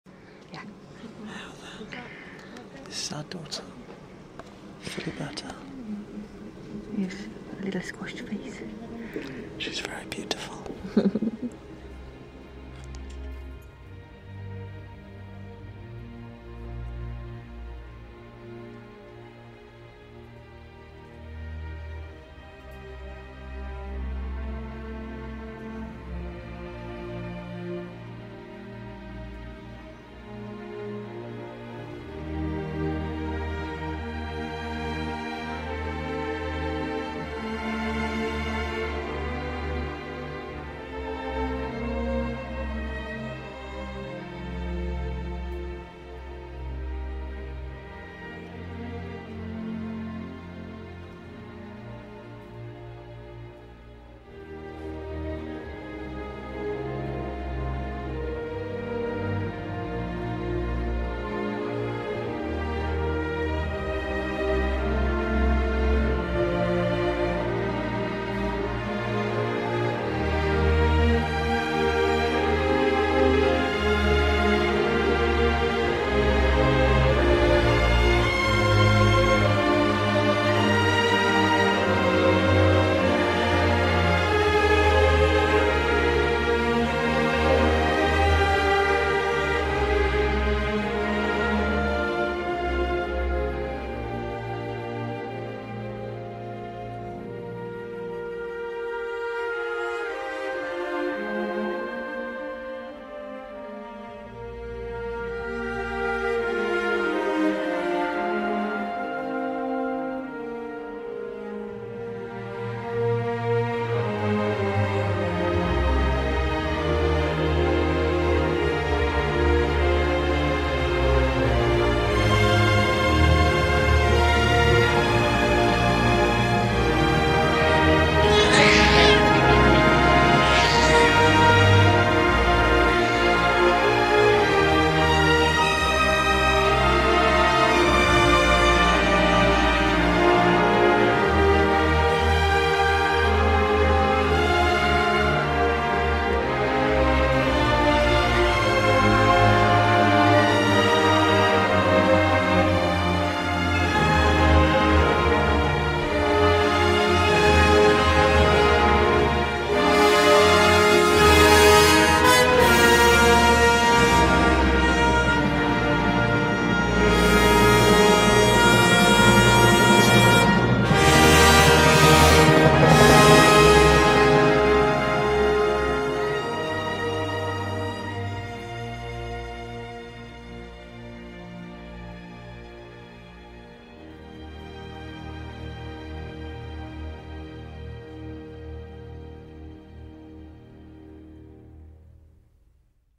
Our baby has arrived...
Our beautiful daughter, Rosa Elizabeth Tyley (for a while provisionally nicknamed  'Philiberta' - an old German name meaning 'very bright!)

The climax of this film shows Philippa soon after the birth, meeting her baby for the first time...